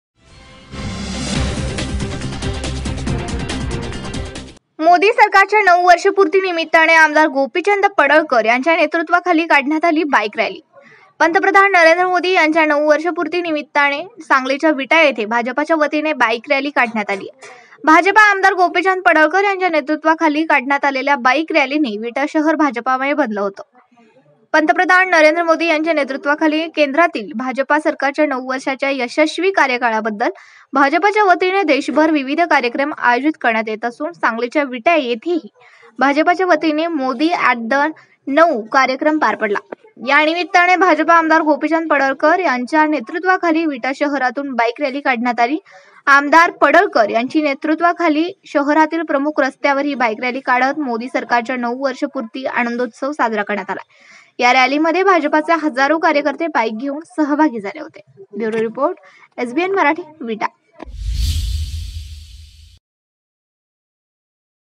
VITA | मोदी सरकारच्या 9 वर्षपूर्ती निमित्ताने आ. पडळकर यांच्या नेतृत्वाखाली काढण्यात आली बाईक रॅली
मोदी सरकारच्या 9 वर्षपूर्ती निमित्ताने आमदार गोपीचंद पडळकर यांच्या नेतृत्वाखाली काढण्यात आली बाईक रॅली